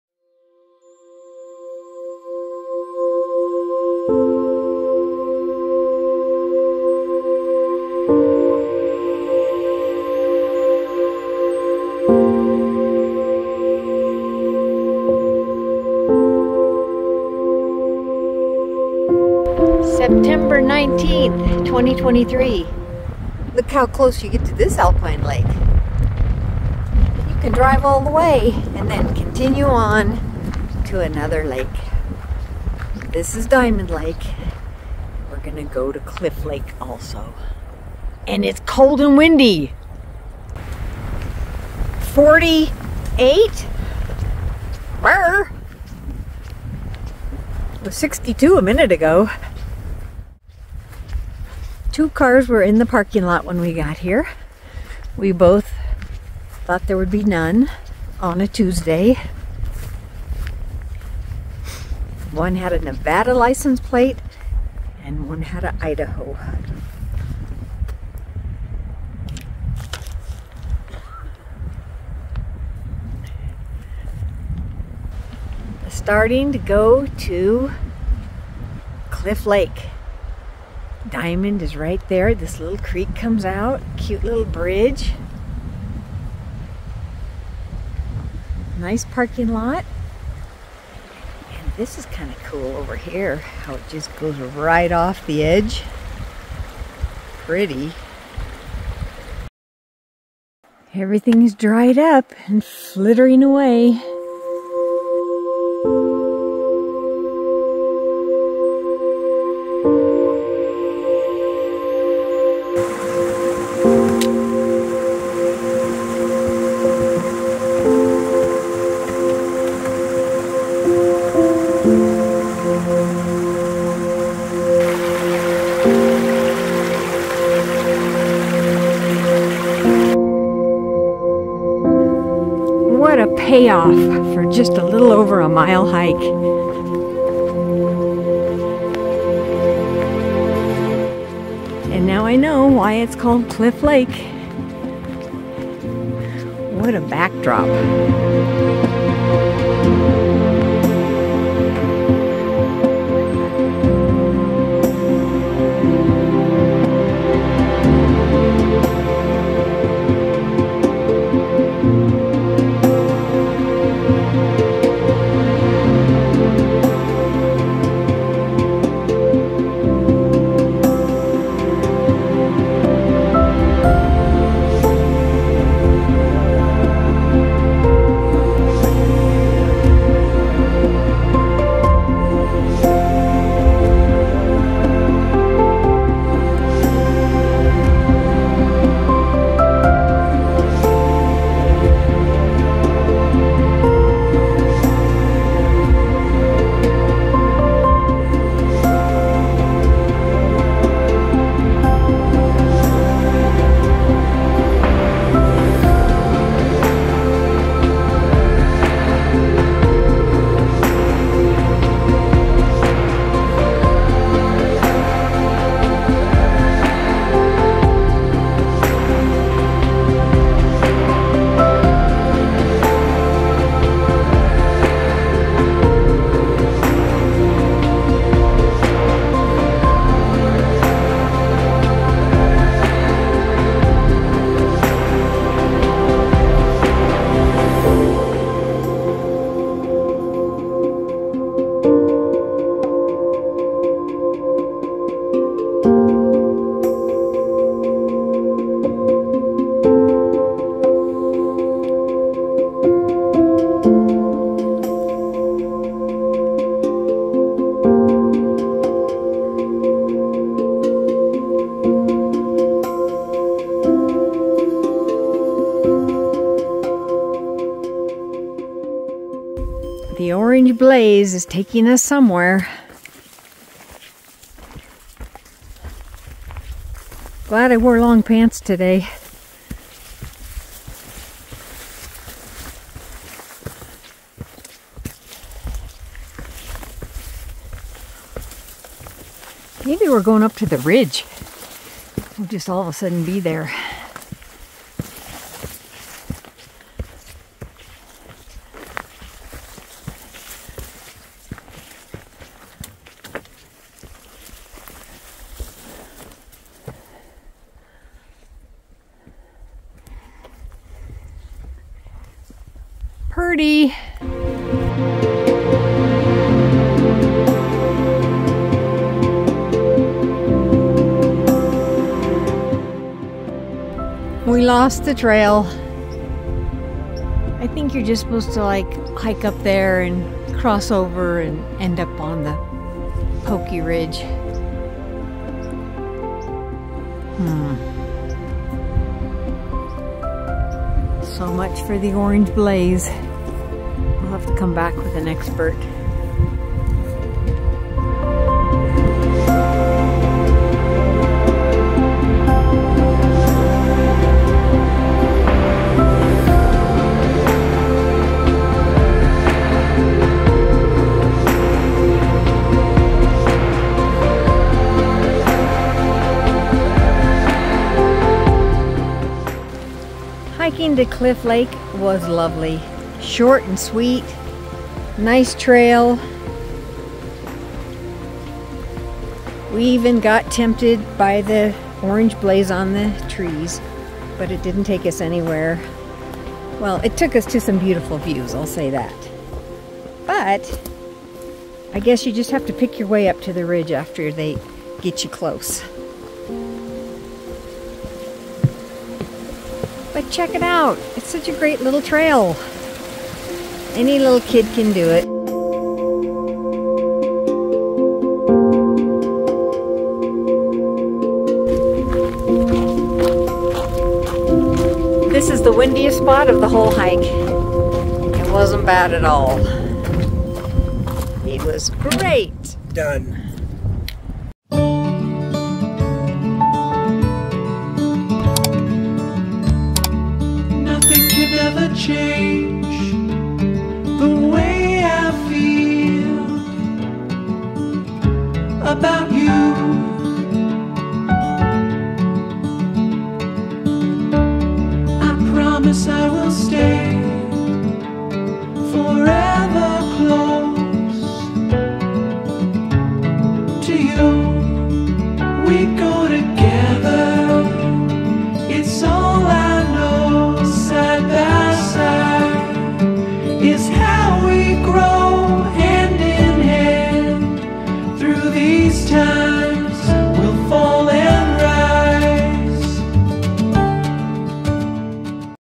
Diamond & Cliff Lakes - Montana Beautiful!
Long drive but a fun short hike to one of Montana's beautiful alpine lakes. We spotted a goat on the cliff at the far end of the lake.